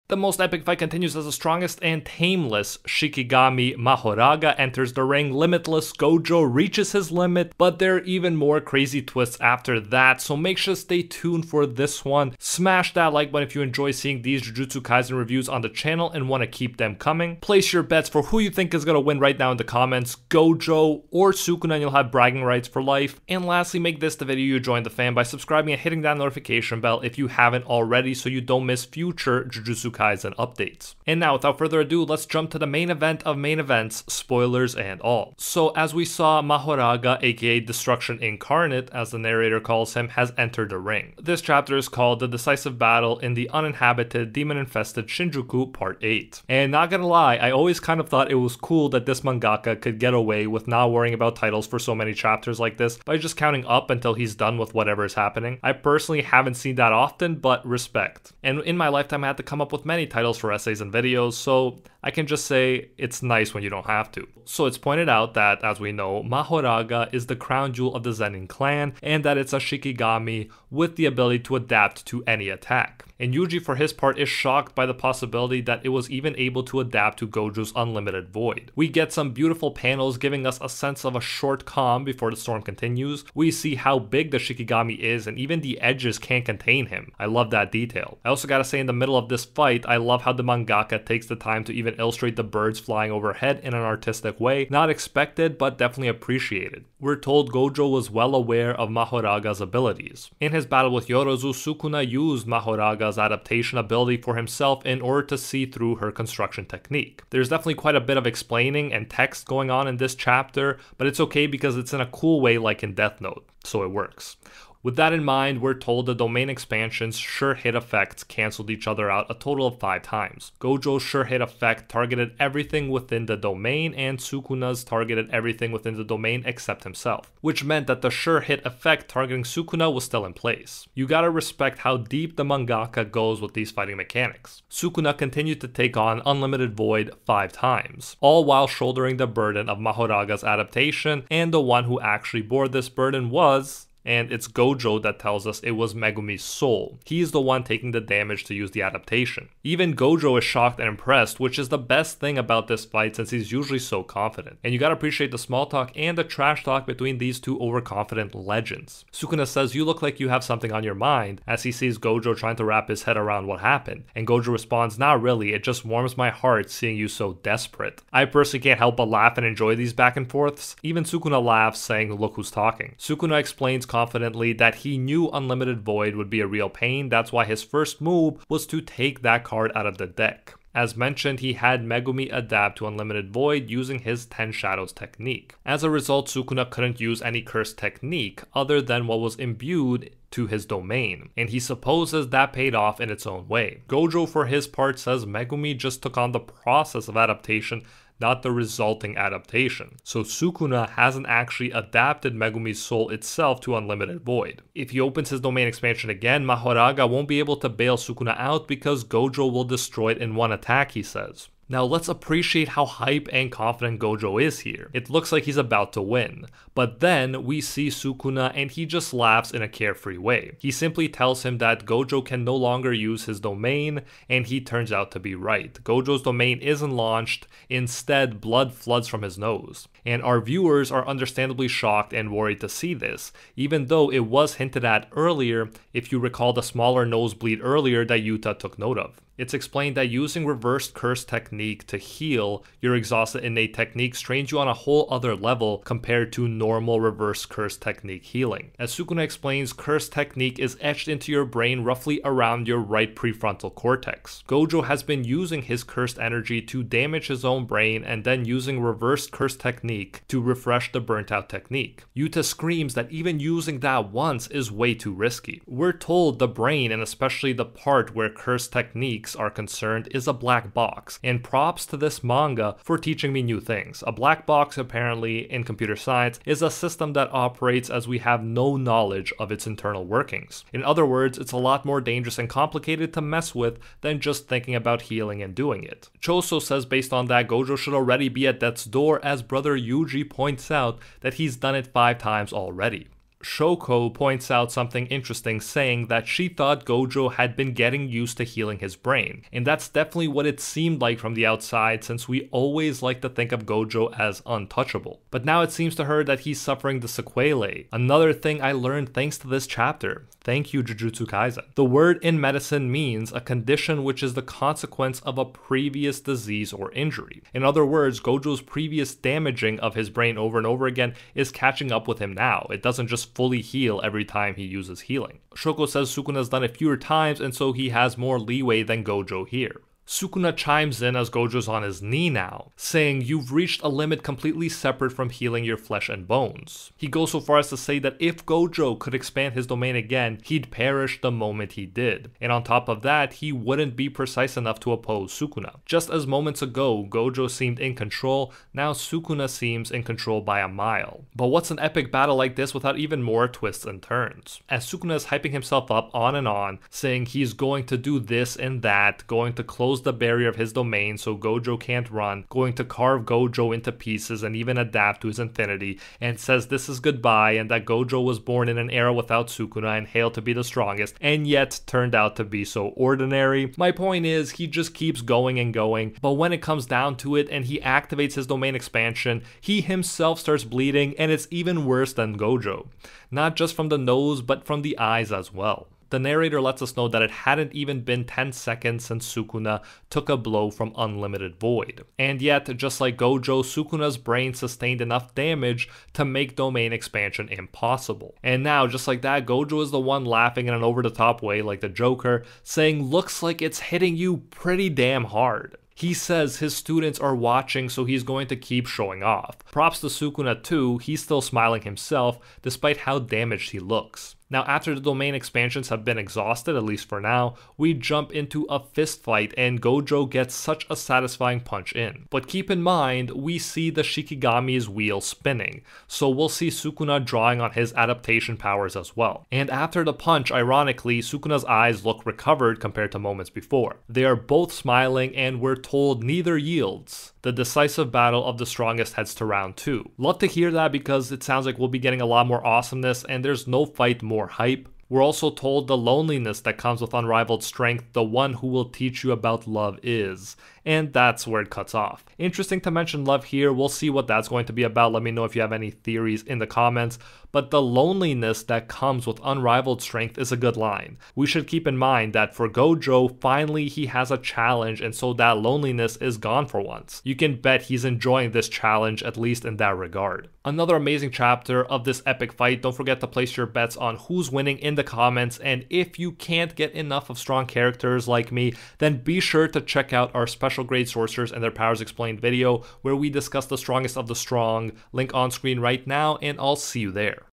GOJO'S NEW TRUE POWER REVEALED!! Jujutsu Kaisen Chapter 230 Reveals Satoru Gojo Power vs Sukuna JJK
Jujutsu Kaisen Chapter 230 reveals Satoru Gojo's secret brain-altering ability to DESTROY Sukuna! Ryomen Sukuna WASN'T READY!

Video by 414anime